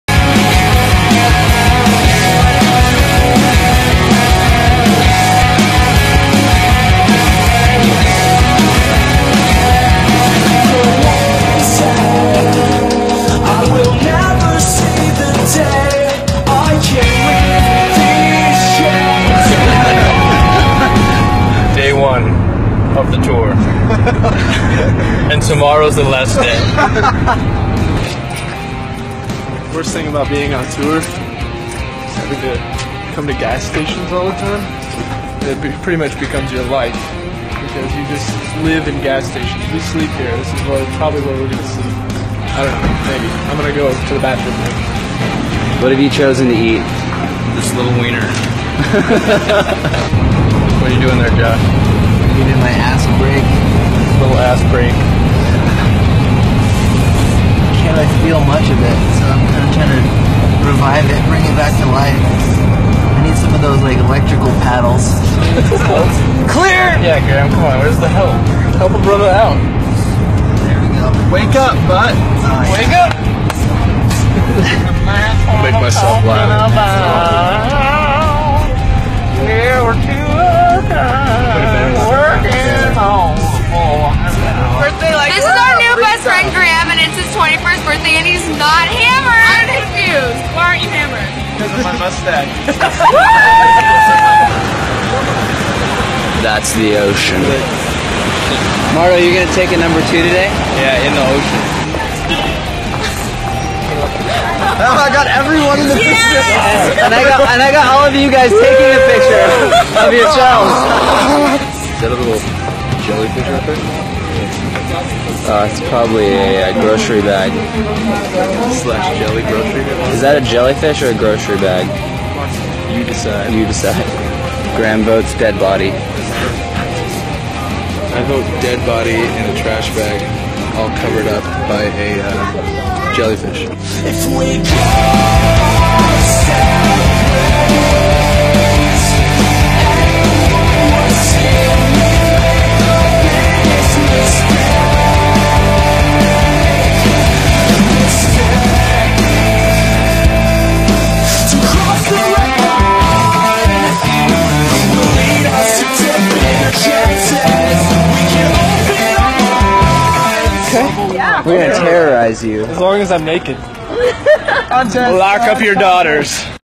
Old Emerald Honor Video 1 (California Tour)
This is an old tour video from a band I was in years ago, fun times. I didn't want the other YT channel that had this video to be the only source of it in case they go away and delete their channel or something, so I have decided to preserve it here.